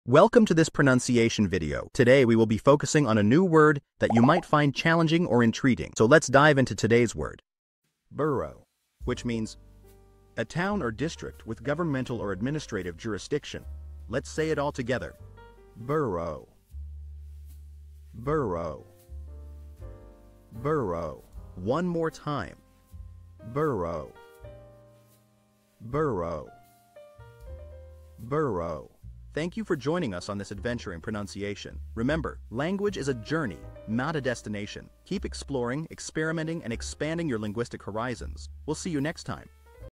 How to pronounce Borough
Master the Pronunciation of 'Borough' - which means : A town or district with governmental or administrative jurisdiction. 📝 with @PPrononciation 🎓🗣️ - [Your Guide to English]

Learn to pronounce 'Borough' perfectly in English with our easy and engaging tutorial by @PPrononciation! 📖📽️ In this comprehensive video, we provide you with native speaker examples 🗣️👂 and phonetic breakdowns 🎵 to ensure you get the pronunciation just right.

How to say Borough in English